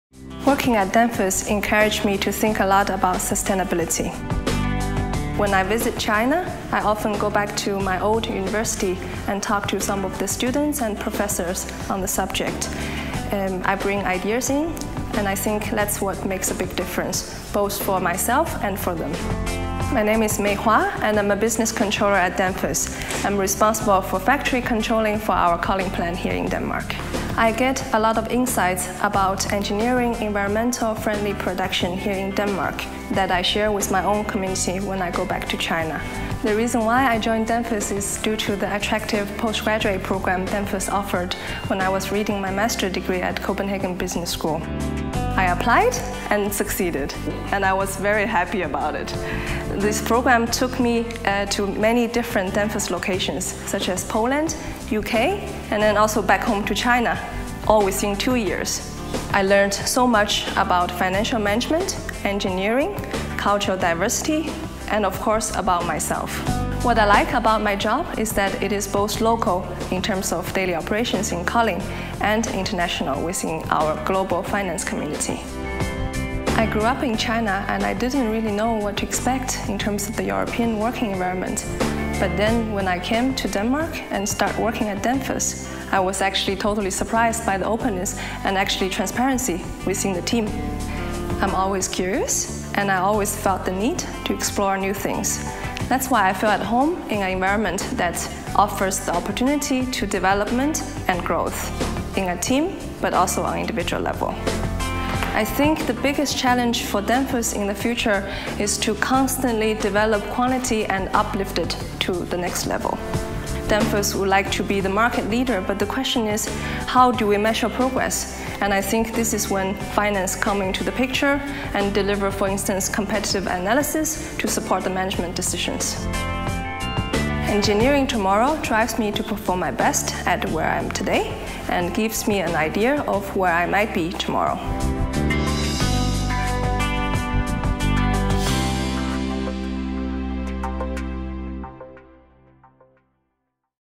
Employer Branding Video - Mei - Business Controlling
5 Employer Branding Videos we have produced/postproduced on behalf of TMP Worldwide Germany GmbH for Danfoss A/S. Key point of this production was showing the employees as authentically als possible in their working environments. The Employer Brand key themes were closely connected with the individual stories and characters of the testimonials. The statements were directly captured during the interview.

Title: Mei - Controlling

Equipment: 2 Panasonic GH4 / 4k production
Locations: Denmark
Concept/Treatment: Beposed, Hamburg
Film production: impossible films GmbH, Hamburg
Director: Gunnar Wittig
Postproduction: Picture Front GmbH & Co. KG Hamburg/München
DOP: Felix Martens
Editing: Svend Minkus
Postsupervision / 3D: Nico Feindt